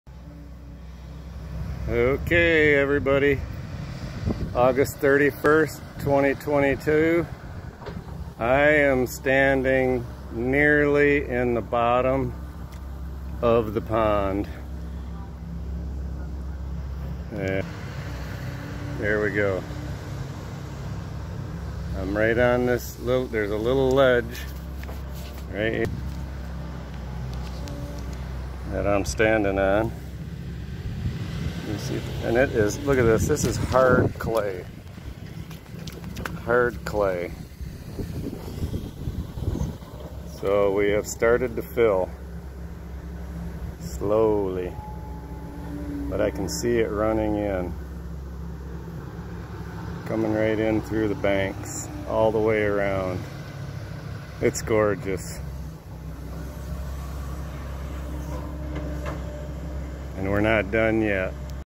pond deep spot
The day we dug our my Grampa Hubert Bachelors old pond from the early 50's.  More to come!!